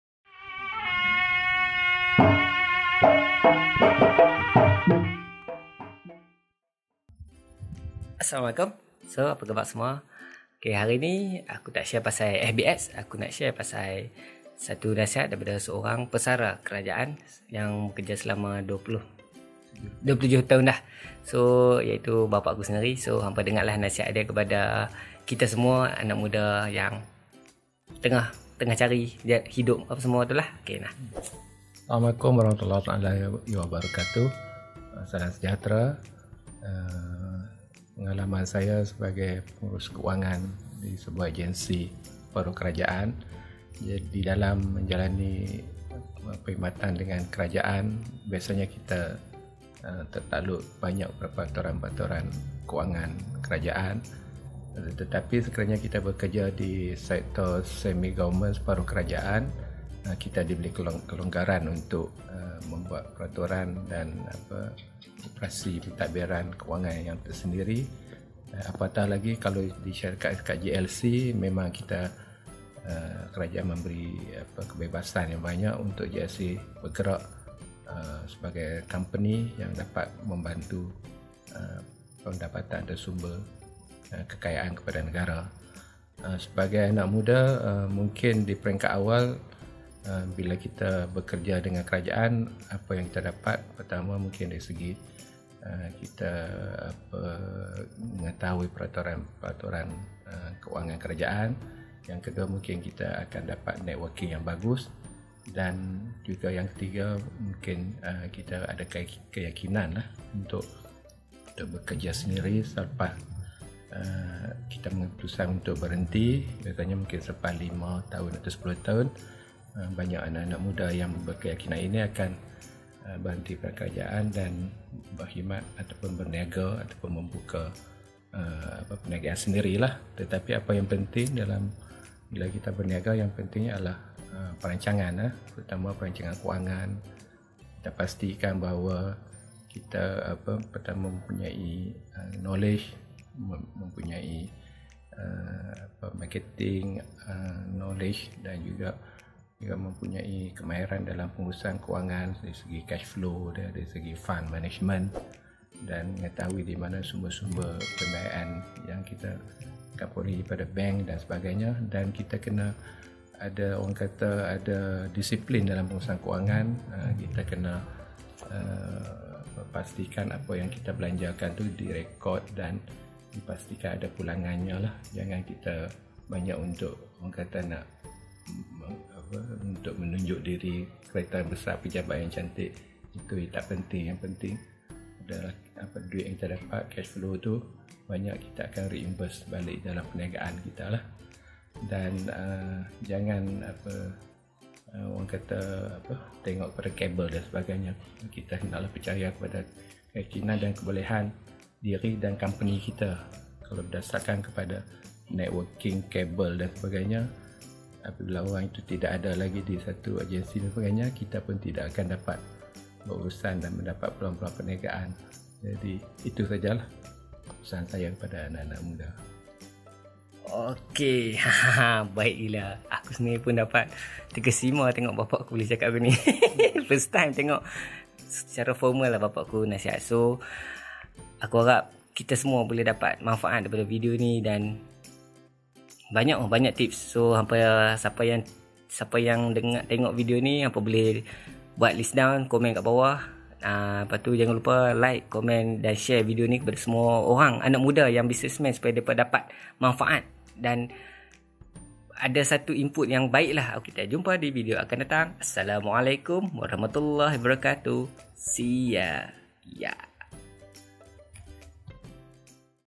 Cara cara hendak mulakan bisnes nasihat dari Bapak
Video ini KHAS buat anak muda yang bekerja dengan Kerajaan,Semi Kerajaan dan ingin buat bisnes sendiri.
Terbaik betoi nasihat dari bapak yasin ni. Yes ini bapak yasin. Inilah en Amron. Yang banyak memberi nasihat,dorongan dan doa untuk yasin selama ini.
Hari ini adalah ulang tahun ke-63 buat bapak yasin. Tolong doakan bapak yasin dipermudahkan segala urusan,dimurahkan rezeki,diberikan sihat tubuh badan dan sentiasa dibawah lindungan Redha Illahi.
Terima kasih bapak kerana membesarkan yasin dengan sempurna. Video ini khas buat bapak semoga ramai anak muda dapat manfaat dari nasihat bapak.
I love you,bapak.

Editing: Adobe Premeire Pro 2020
Camera: Dji Osmo Pocket
Drone:Mavic Mini
External Mic:Rode